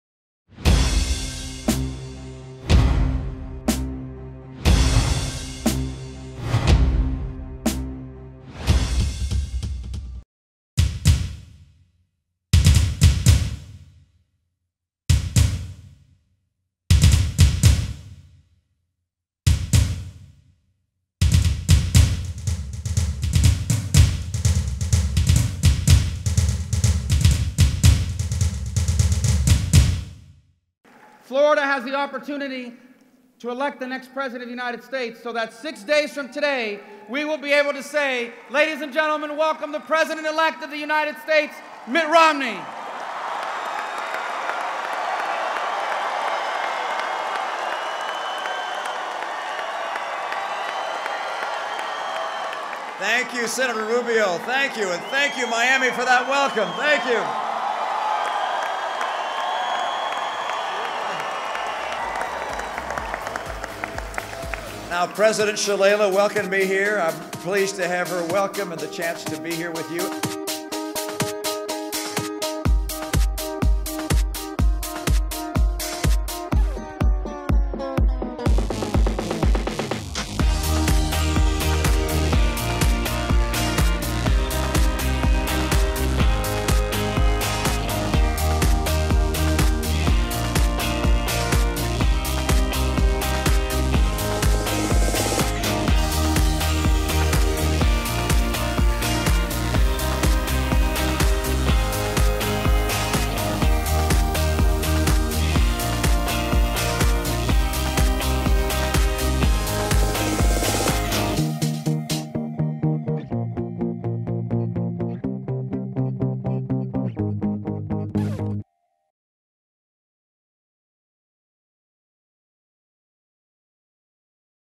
Presidential Candidate Mitt Romney at University of Miami for Election Rally
Romney's "Victory Rally" on Wednesday gave UM employees and students a chance to hear not only from Romney but other prominent figures in the Republican Party. U.S. Representative Ileana Ros-Lehtinen, who earned her Ph.D. from UM, spoke prior to Romney, as did U.S. Senate candidate Connie Mack, former Florida Governor Jeb Bush, and Florida Senator Marco Rubio, a Miami Law graduate.